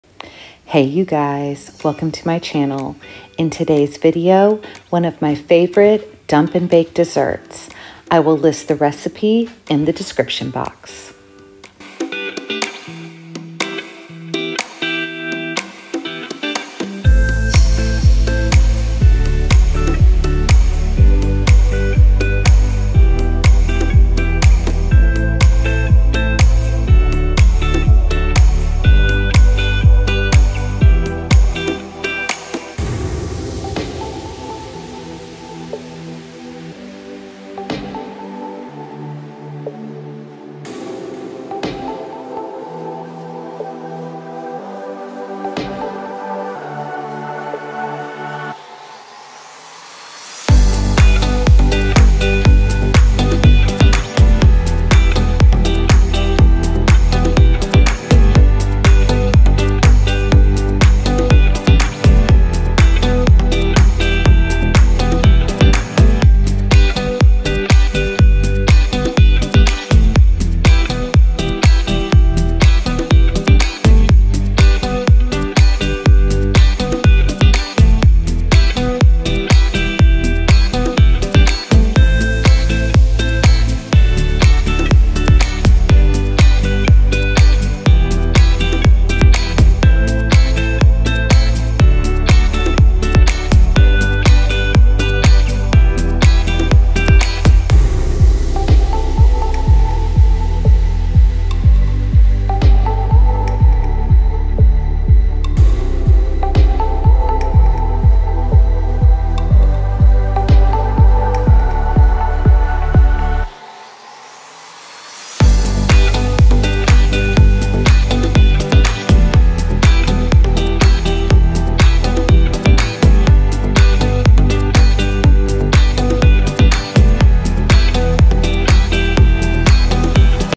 EASY! Dump & Bake | Berry Crisp | 5 Ingredients
This EASY dump & bake dessert takes
only 5 ingredients! Perfect for upcoming
Spring & Summer gatherings. Can be
served warm or at room temperature.

🍓 Dump & Bake - Berry Crisp -

2 - 16oz - bags frozen mixed berries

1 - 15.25oz - yellow cake mix
( I used Betty Crocker Butter Recipe )

1 - stick butter

1/3 cup - rolled oats

1/3 cup - chopped walnuts

Optional - whipped cream or
vanilla ice cream for serving

🍓 Instructions -

Grease a 9x13 in baking dish with non-stick
cooking spray or butter.  Dump the frozen
berries into the prepared pan. Evenly place
the cake mix over the berries. Top the cake
mix with the oats, and walnuts.  Melt the butter
and drizzle over the top. Bake at 350
degrees F for 50 to 60 minutes. Top
will be lightly golden brown. Serve
warm or at room temperature.